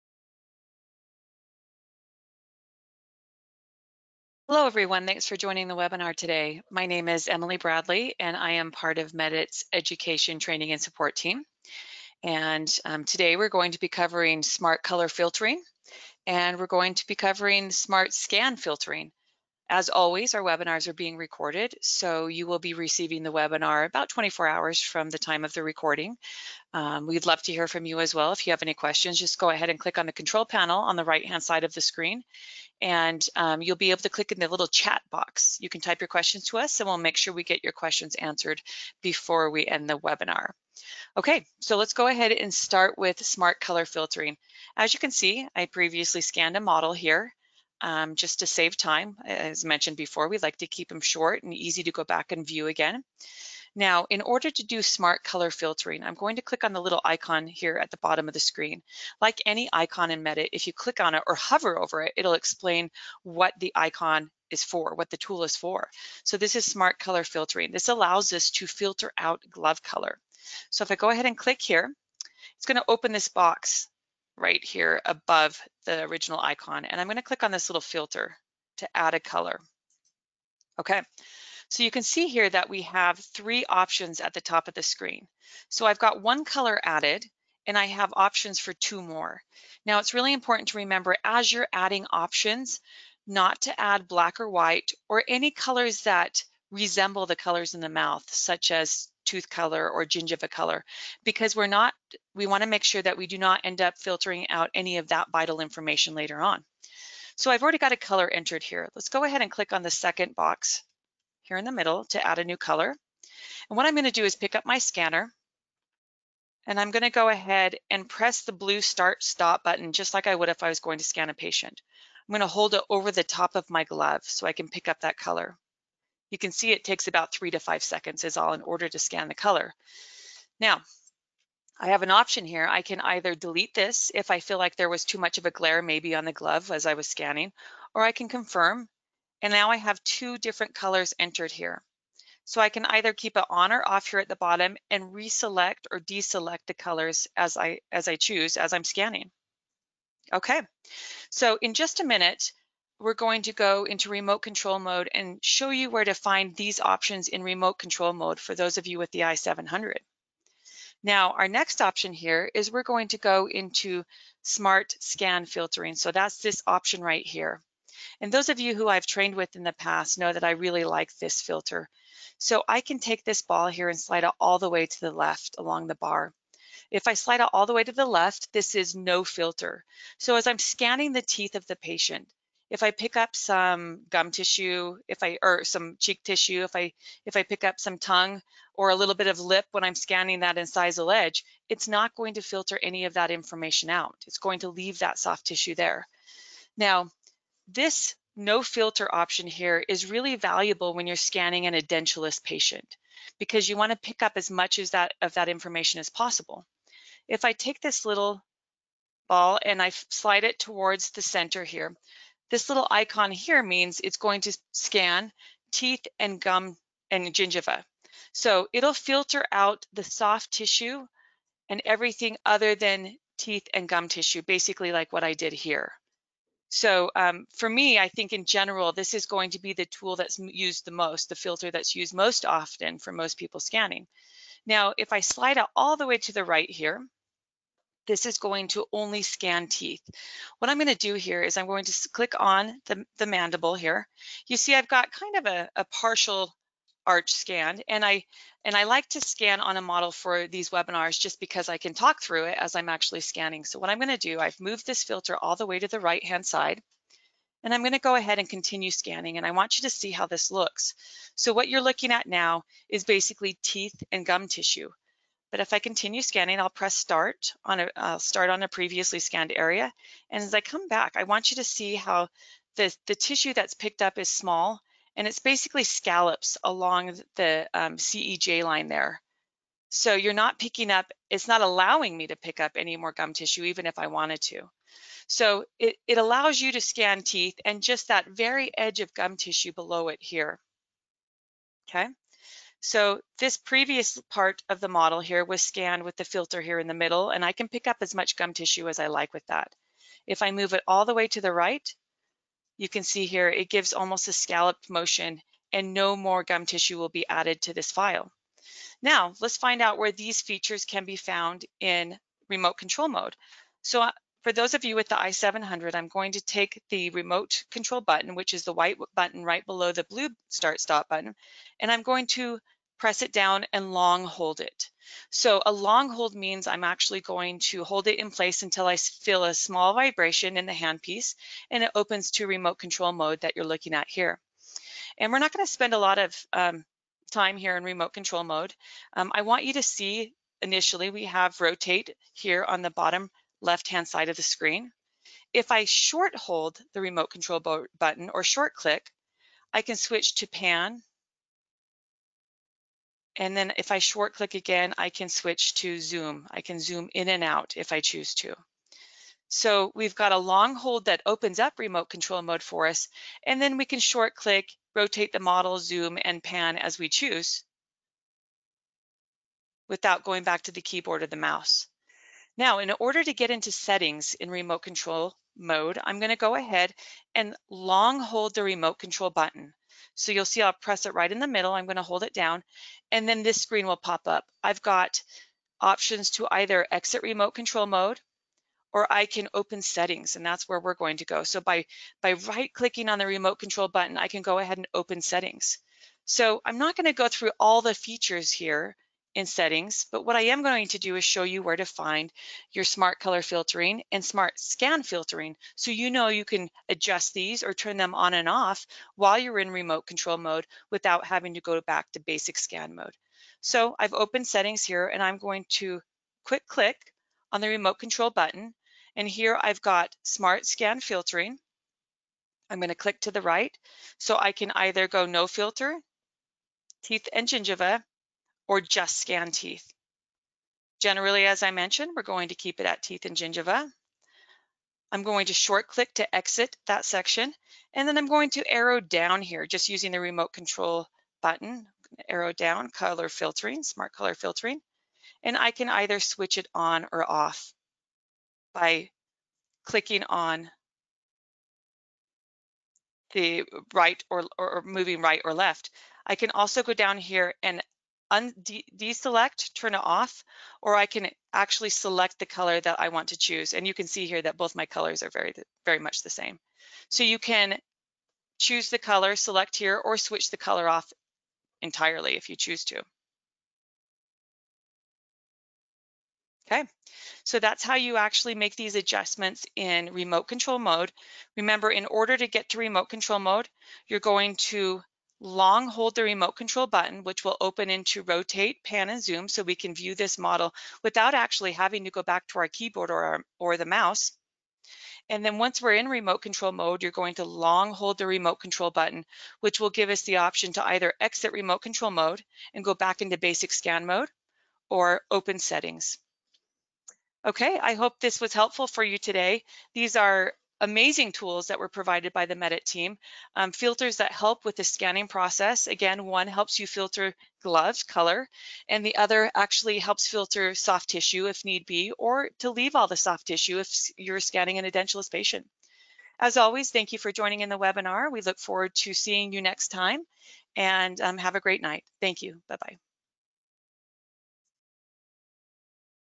Medit Webinar - Smart Scan Filtering & Smart Color Filtering (Pre-recorded)
Learn more about Smart Scan Filtering and Smart Color Filtering in Medit Scan for Clinics.
You will see the registered color filtered from the scan in real-time.
Also, Smart Scan Filtering will remove soft tissue such as lips, cheeks, and tongue and reduce your future editing time.